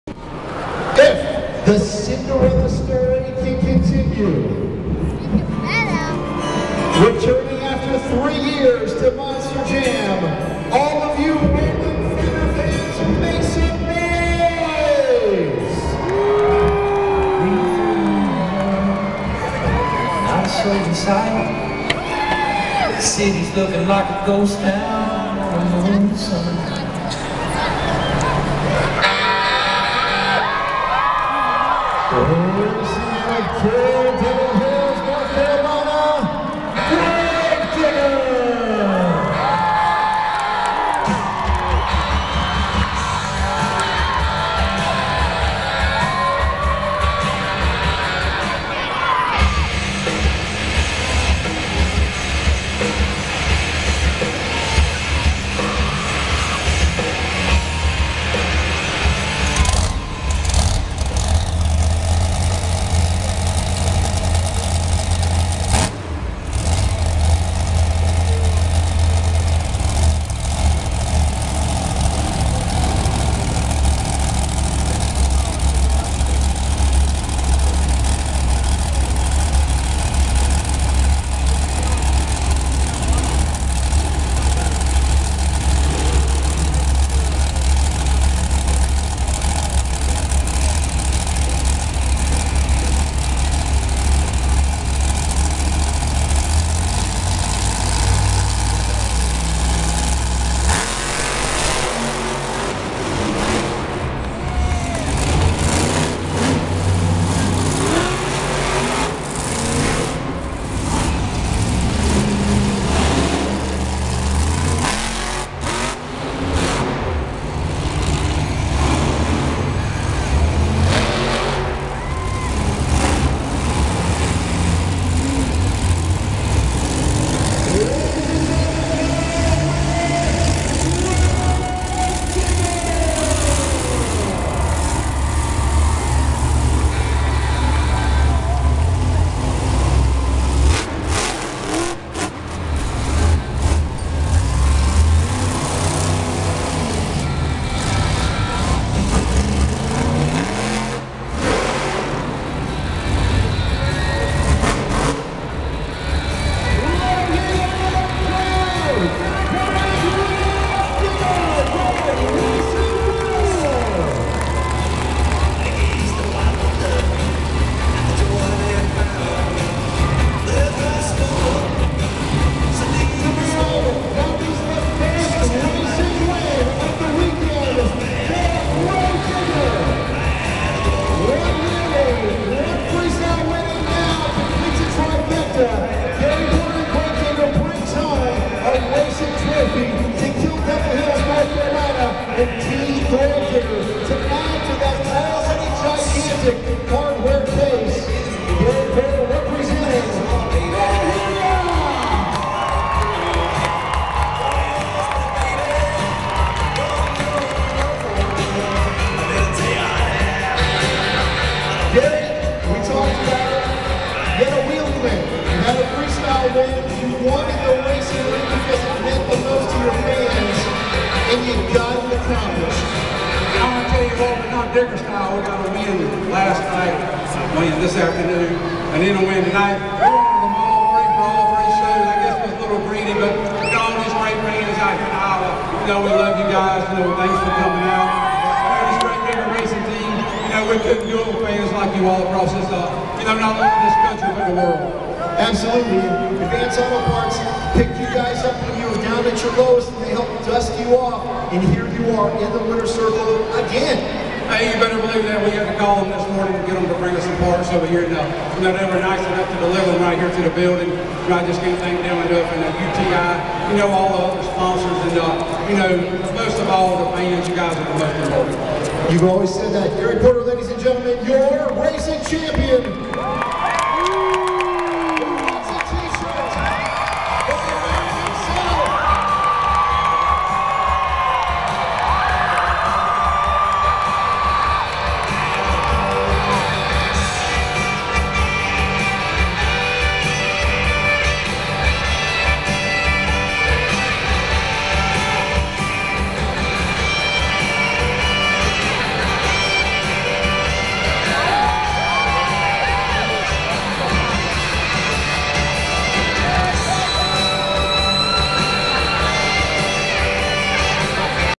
Grave Digger & Rollin Thunder finals Saturday night Des Moines 2013
Grave Digger & Rollin Thunder finals Saturday night Des Moines 1-5-2013  Grave Digger WINS